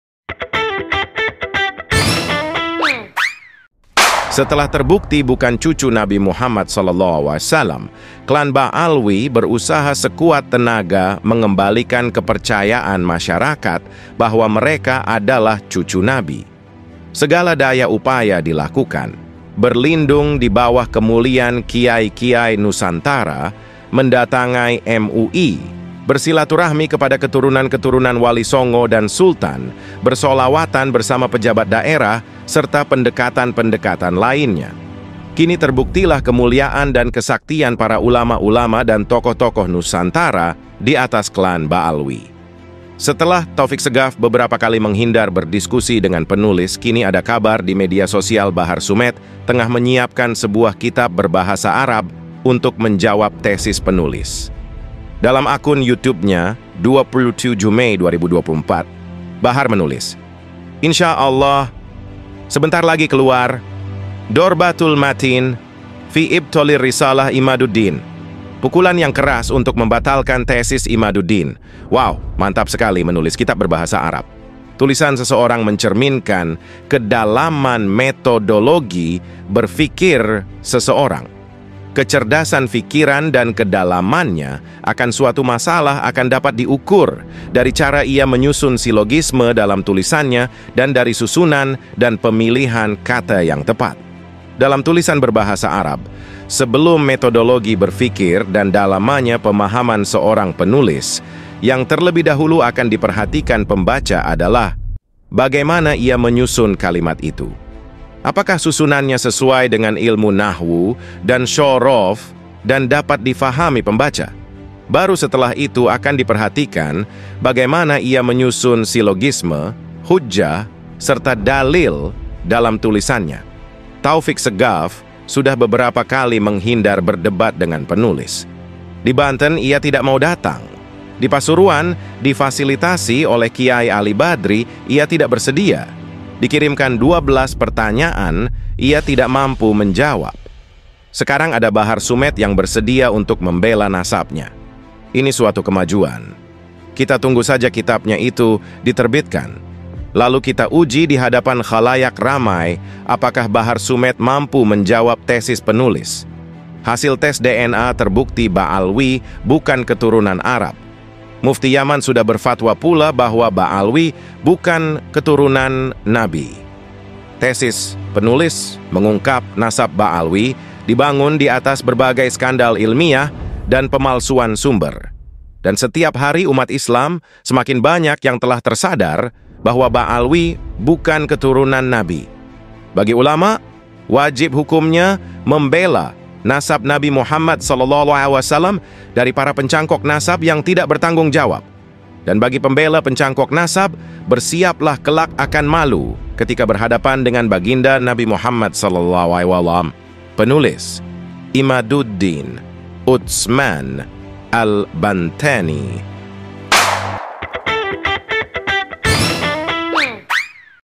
BAHAR SIAPKAN KITAB ANTI TESIS!! INI TANGGAPAN KIAI IMAD!
Terbaru. ada kabar tentang Bahar Smit karang kitab anti tesis, ini kata Kiai Imad!